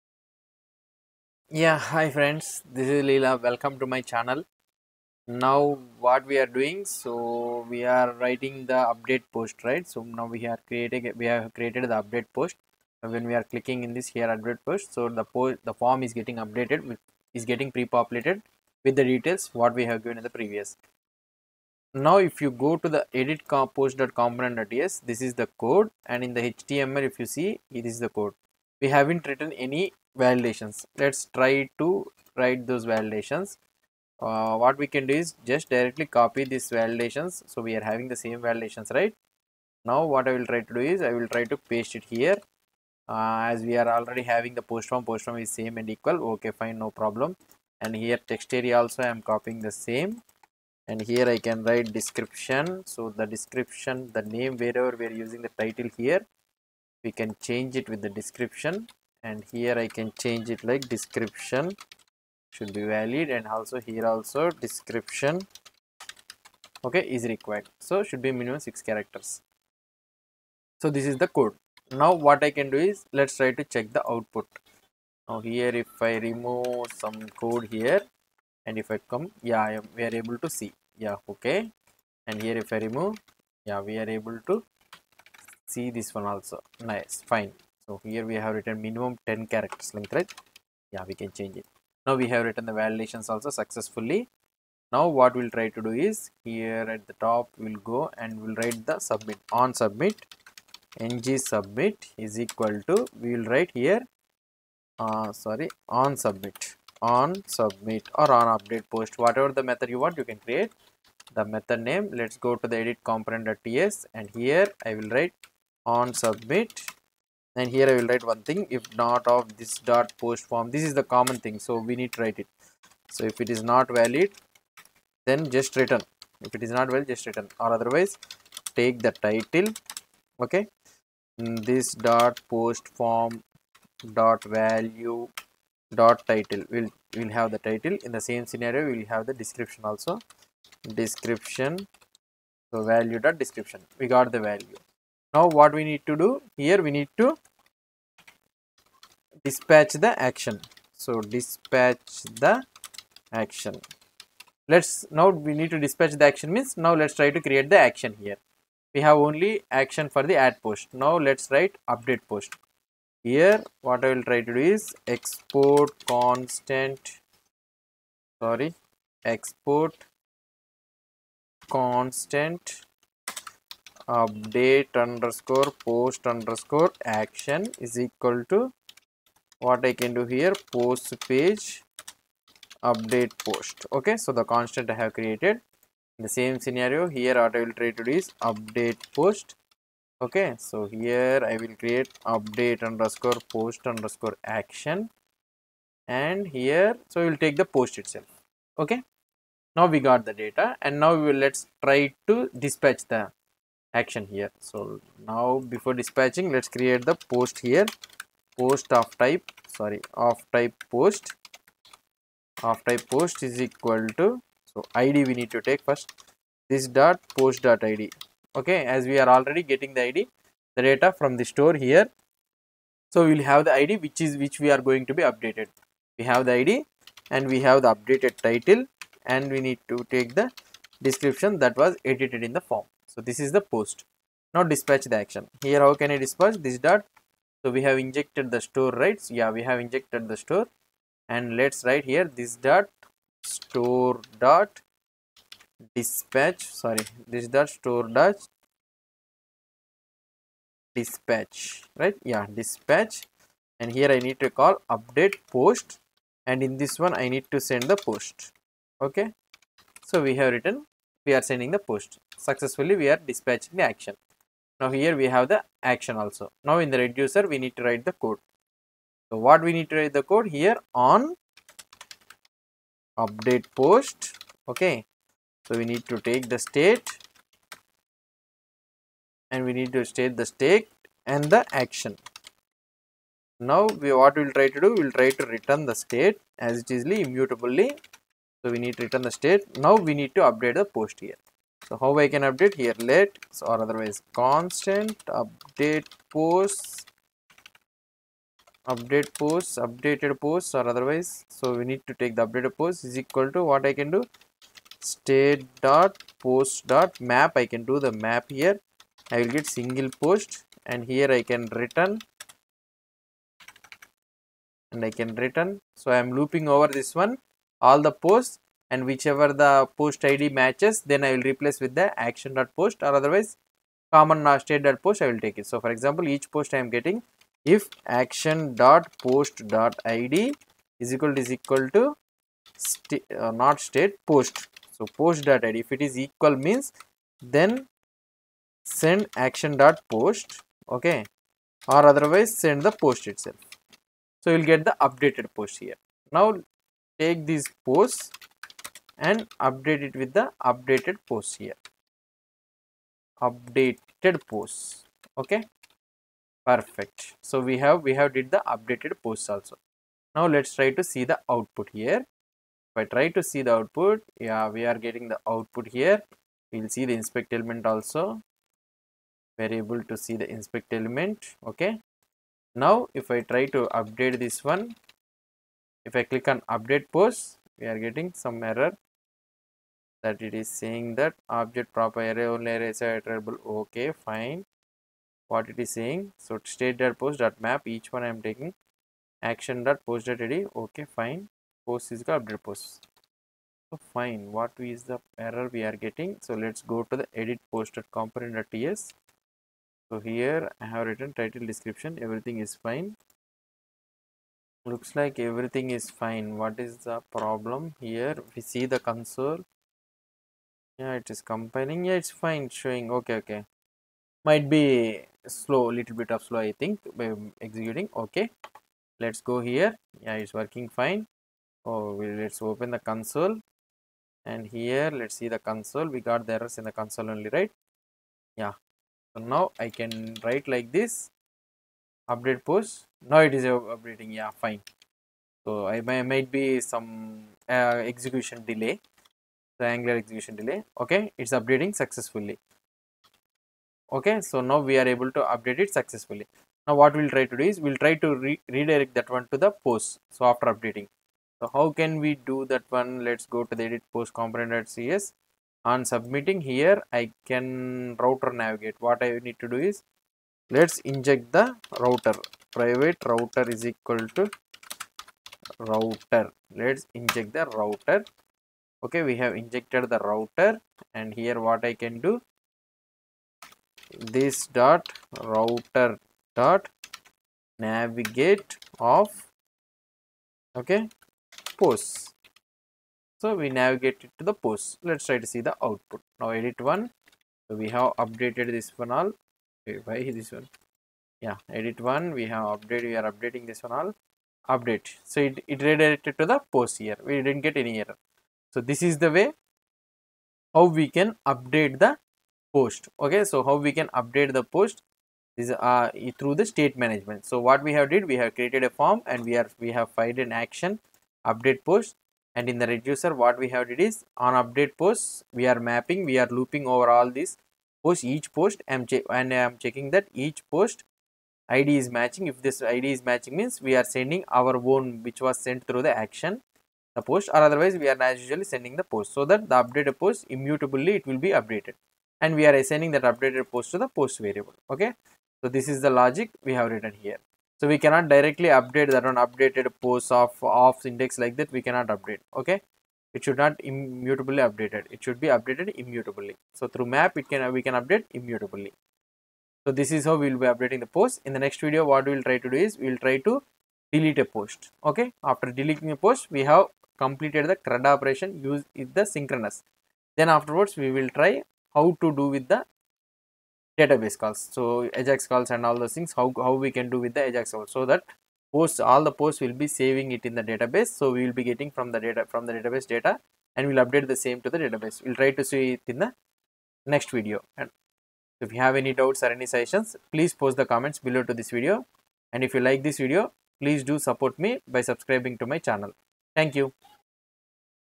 18. Update Post in Ngrx store immutable with actions in Angular Ngrx
Hi Friends
In this video, we will see how to update the post data in the ngrx store state immutable in angular ngrx